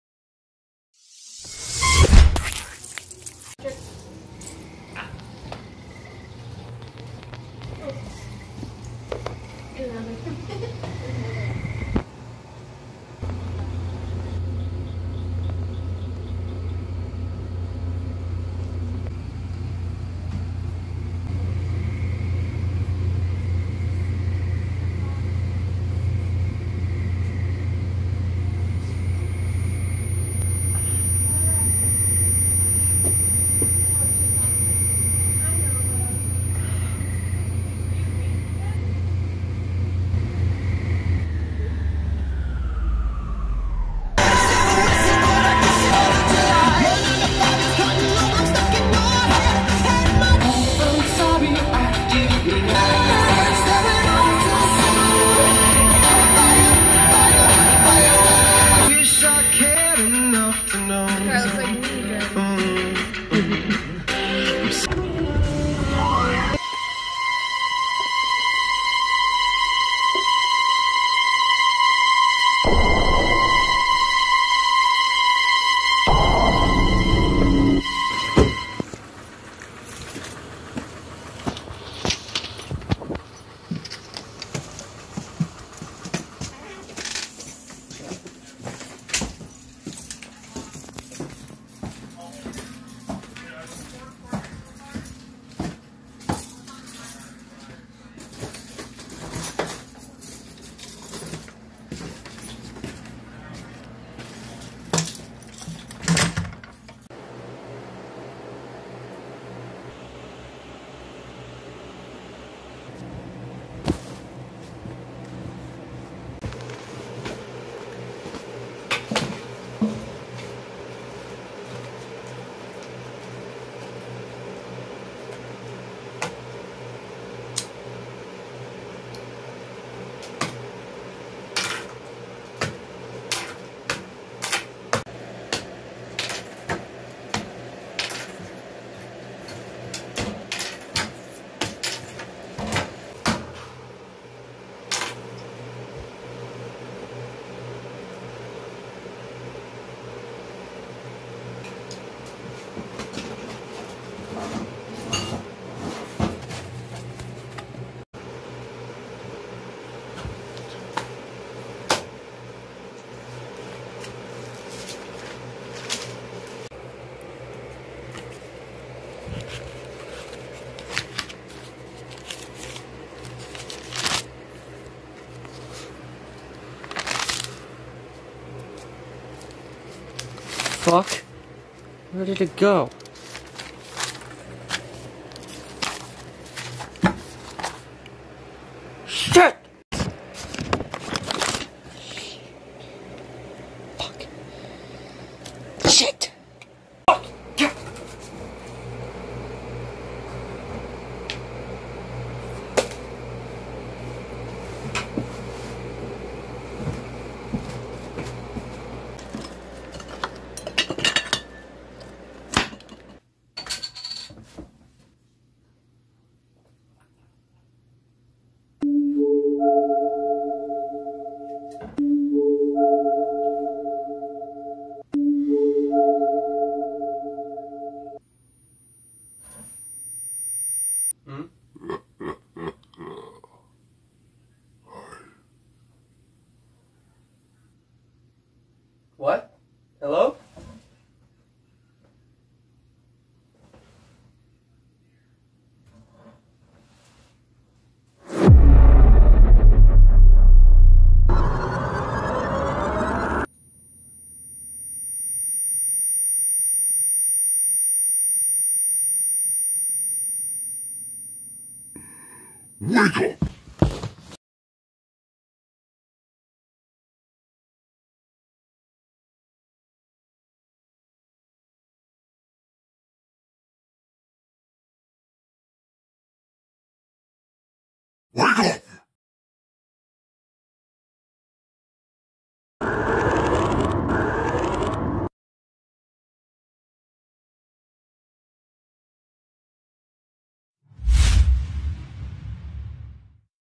NIMRØD part 1 by//gavin Lee
a writer battles with schizophrenia and alcoholism as he gets home and finds a peice of material missing...